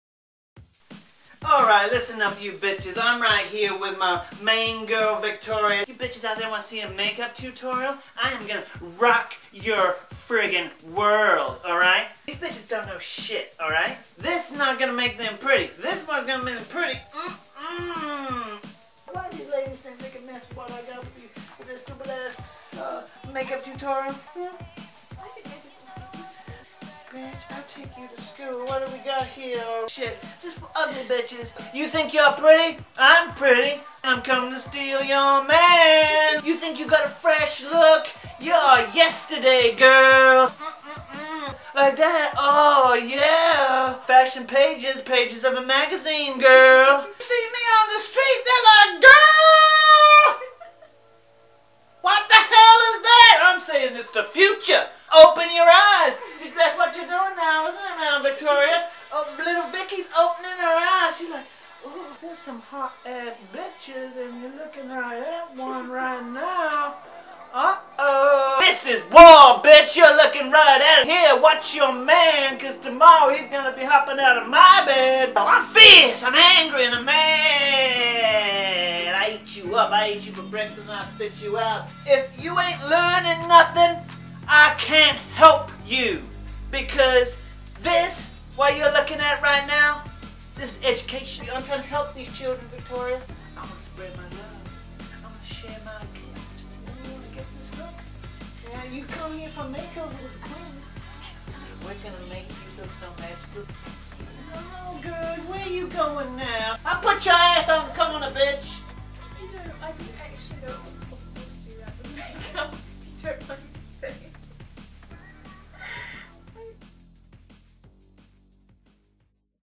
Make Up Tutorial 1
You ain't ready for the DJ Romantech Make Up Tutorial. Girl I'm sorry but you are yesterday. It's time for a new look that's fierce and fashion forward, honey.